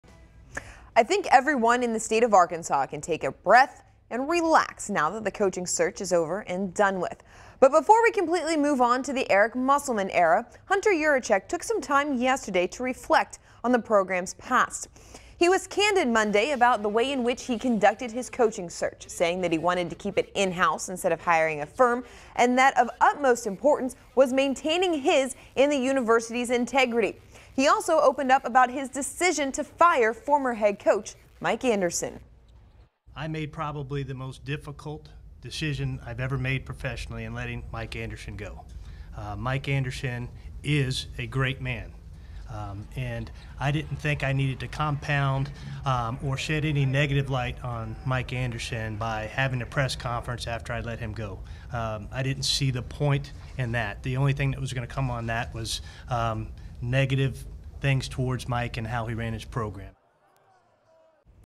Hunter Yurachek on decision to fire Mike Anderson
The Arkansas athletic director got candid Monday, calling the decision to fire Mike Anderson, "the most difficult of his professional career."

Cover image provided by Associated Press